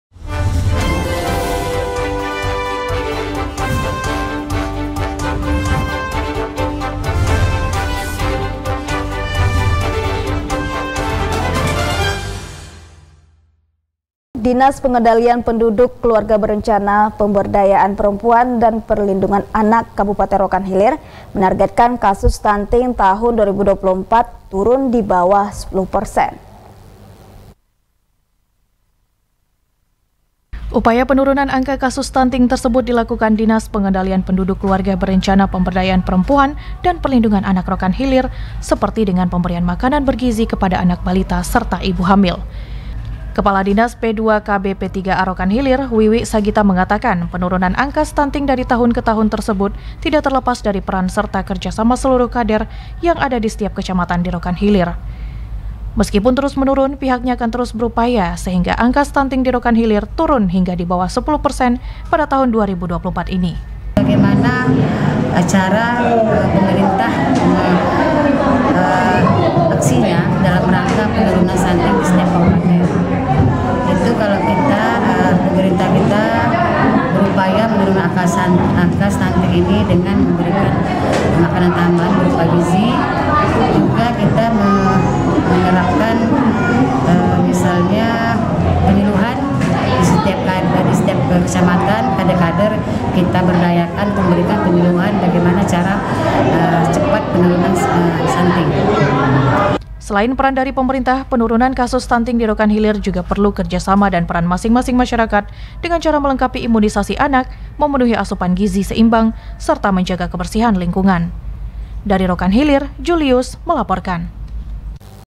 TAHUN 2024, ANGKA STUNTING DITARGETKAN TURUN DI BAWAH 10 PERSEN
Rokan Hilir, riautelevisi.com- Dinas pengendalian Penduduk Keluarga Berencana Pemberdayaan Perempuan dan Perlindungan Anak Kabupaten Rokan Hilir menargetkan kasus stunting tahun 2024 ini turun di bawah 10 persen.

Upaya penurunan angka kasus stunting tersebut dilakukan Dinas Pengendalian Penduduk Keluarga Berencana Pemberdayaan Perempuan dan Perlindungan Anak Rokan Hilir seperti dengan pemberian makanan bergizi  kepada anak balita serta ibu hamil. Kepala Dinas P2KBP3A Rokan Hilir, Wiwik Sagita mengatakan,  penurunan angka stunting dari tahun ke tahun tersebut tidak terlepas dari peran serta kerjasama seluruh kader yang ada di setiap Kecamatan di Rokan Hilir. Meskipun terus menurun, pihaknya akan terus berupaya sehigga angka stunting di Rohil turun hingga di bawah 10 persen pada tahun 2024 ini.

Selain peran dari pemerintah, penurunan  kasus stunting di rohil juga perlu kerjasama dan peran masing-masing maysarakat dengan cara melengkapi imunisasi anak, memenuhi asupan gizi seimbang serta menjaga kebersihan lingkungan.